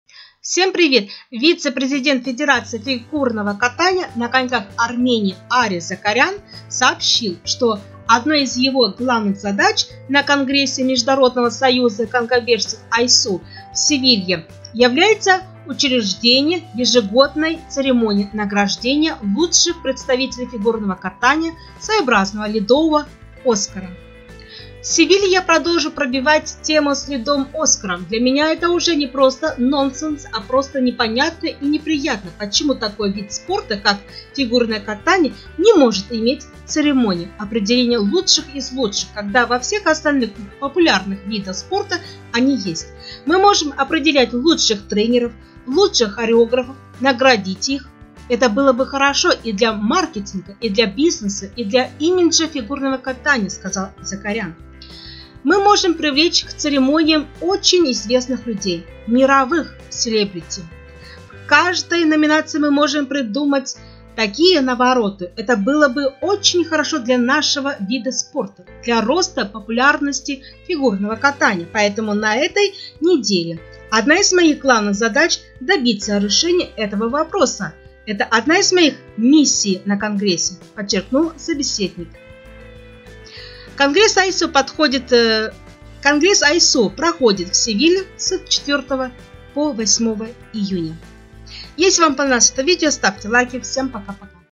Закарян: нонсенс, что в фигурном катании нет церемонии награждения лучших из лучших
11) Брайан Орсер: «Не ожидаю, что все мои ученики будут заниматься на льду одновременно. У нас есть другой каток, так что все будет управляемо»
9) Юдзуру Ханю: «Я провожу время в отеле, поэтому к сожалению не могу побывать в других местах»